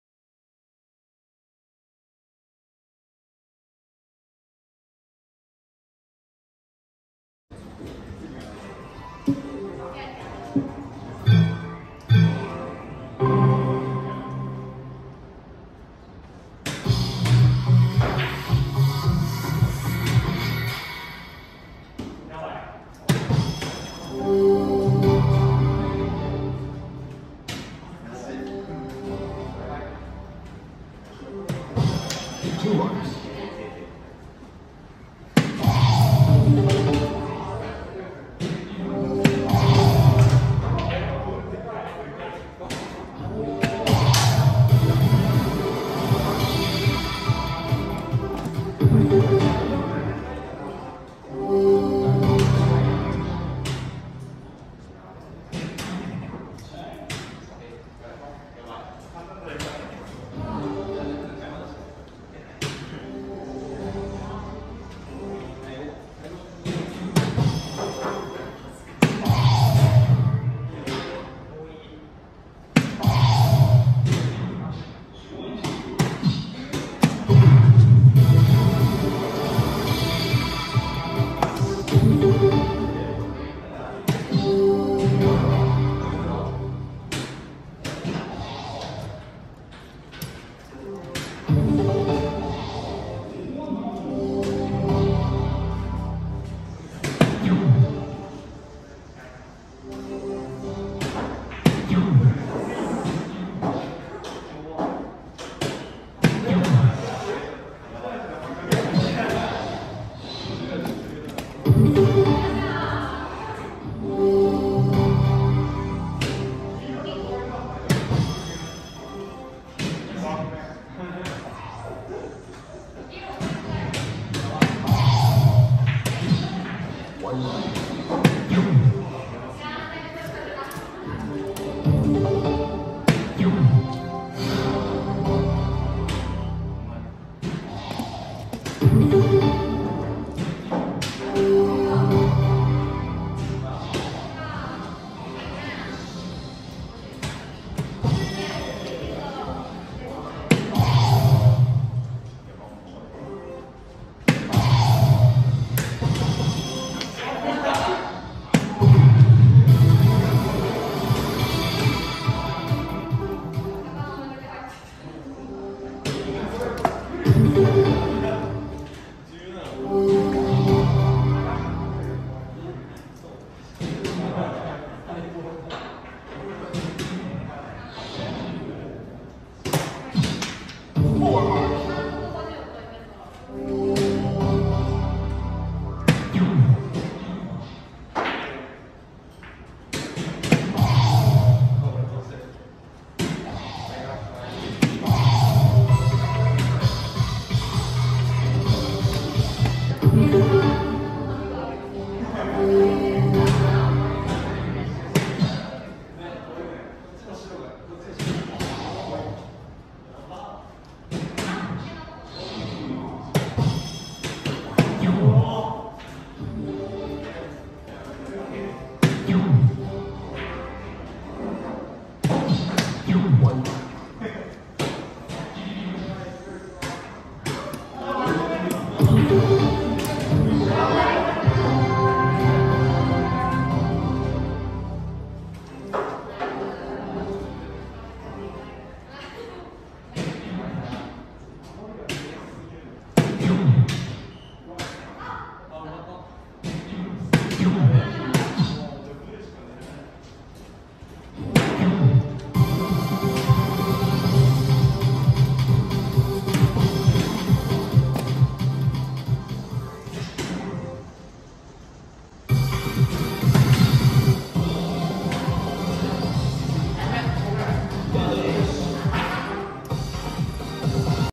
【ﾀﾞｰﾂ】【目指せ!! 1000over】KENSHIRO-JAPANPRODARTS- #017
【今回のチャレンジ】
▶︎count-upで1000点目指します!!

少しでも応援して頂けたら嬉しいです♪